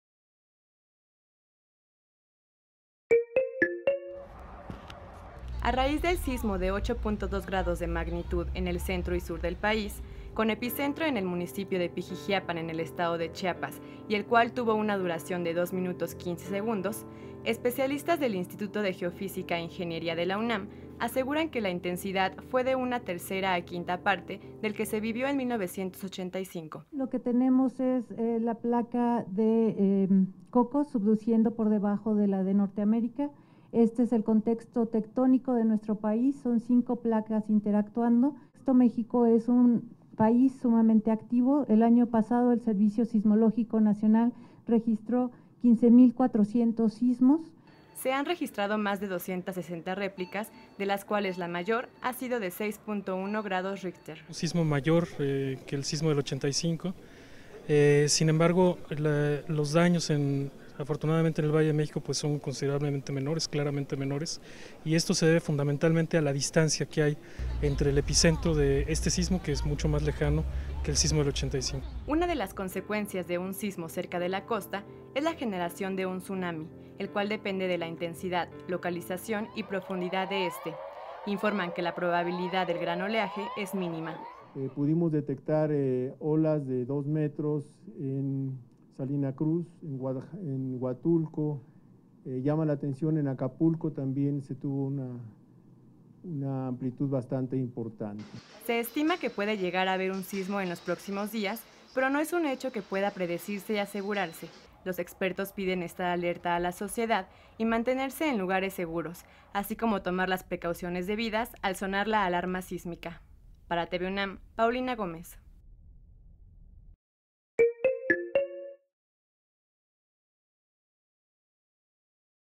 SISMO EN MÉXICO REPORTE DEL INSTITUTO DE GEOFÍSICA UNAM
ESPECIALISTAS DEL INSTITUTO DE GEOFÍSICA E INGENIERÍA DE LA UNAM ASEGURAN QUE LA INTENSIDAD FUE DE UNA TERCERA A QUINTA PARTE DEL QUE SE VIVIÓ EN 1985